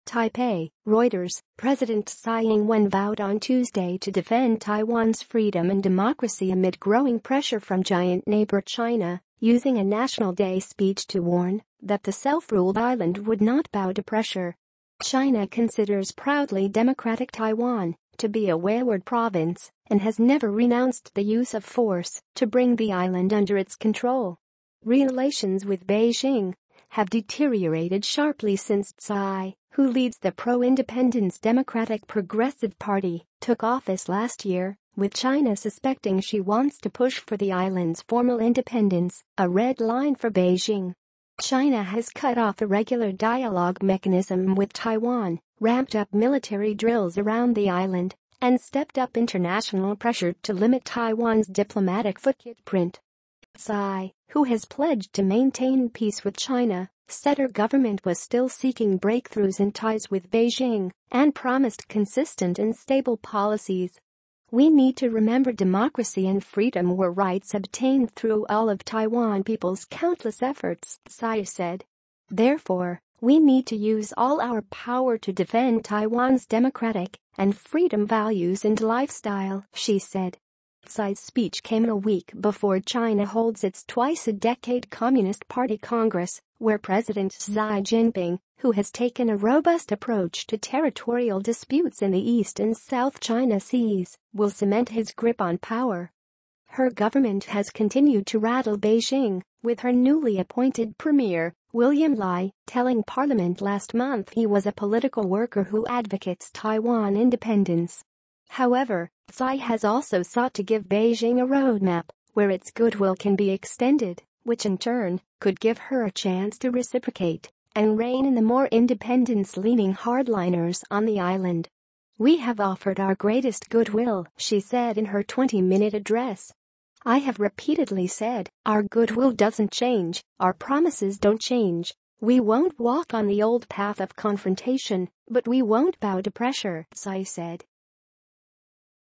Taiwan president pledges to defend freedoms despite China pressure
TAIPEI (Reuters) - President Tsai Ing-wen vowed on Tuesday to defend Taiwan’s freedom and democracy amid growing pressure from giant neighbour China, using a National Day speech to warn that the self-ruled island would not bow to pressure.

China considers proudly democratic Taiwan to be a wayward province and has never renounced the use of force to bring the island under its control.

Relations with Beijing have deteriorated sharply since Tsai, who leads the pro-independence Democratic Progressive Party, took office last year, with China suspecting she wants to push for the island’s formal independence, a red line for Beijing.

China has cut off a regular dialogue mechanism with Taiwan, ramped up military drills around the island and stepped up international pressure to limit Taiwan’s diplomatic footprint.

Tsai, who has pledged to maintain peace with China, said her government was still seeking breakthroughs in ties with Beijing and promised consistent and stable policies.

“We need to remember democracy and freedom were rights obtained through all of Taiwan people’s countless efforts,” Tsai said.

“Therefore, we need to use all our power to defend Taiwan’s democratic and freedom values and lifestyle,” she said.

Tsai’s speech came a week before China holds its twice-a-decade Communist Party Congress, where President Xi Jinping, who has taken a robust approach to territorial disputes in the East and South China Seas, will cement his grip on power.

Her government has contin